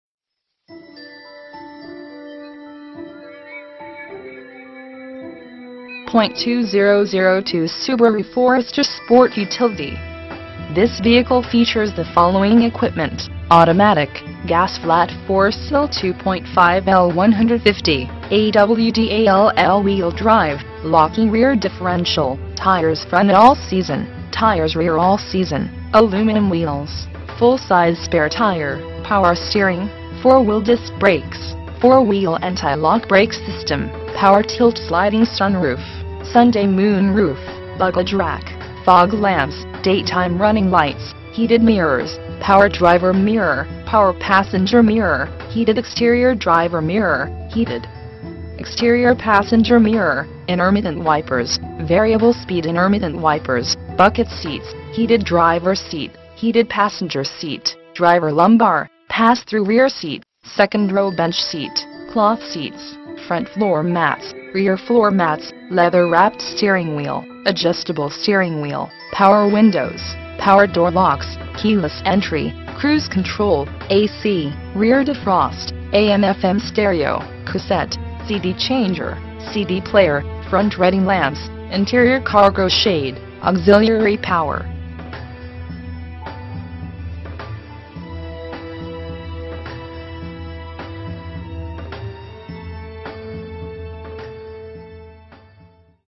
2002 Subaru Forester - Classic Dealer Group
For Sale: 2002 Subaru Forester
VIN: JF1SF65632H758335
Engine: Gas Flat 4-Cyl 2.5L/150
Drivetrain: AWD
Transmission: Automatic
Mileage: 128,687
Color: Aspen White (exterior) Gray (interior)

Features:
2002 Subaru Forester Sport Utility.